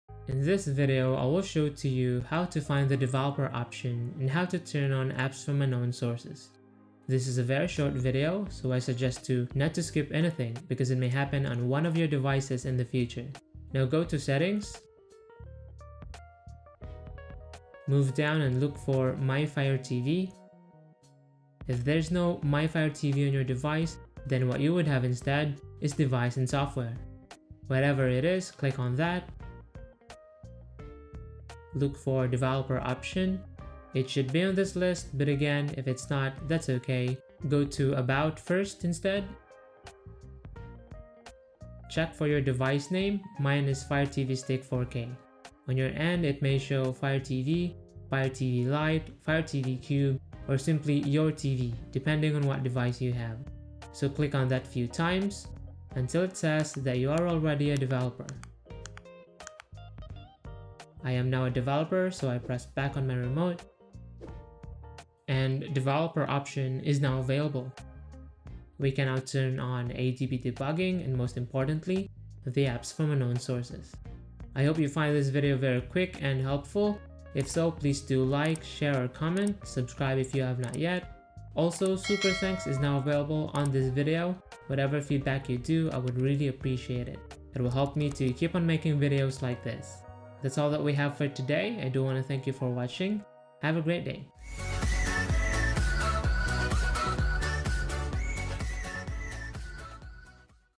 Allow Apps from Unknown Sources (2023) | Amazon Fire TV Guide
How to turn on Apps from Unknown Sources, access My Fire TV and/or Developer Options that are now hidden when you get a new Amazon Fire TV Device or After you made a software update or factory reset.

AMAZON FIRE TV STICK 4K MAX

AMAZON FIRE TV STICK HD

AMAZON FIRE TV STICK  4K

Xiaomi Mi TV Stick

Xiaomi Mi Box S 4K HDR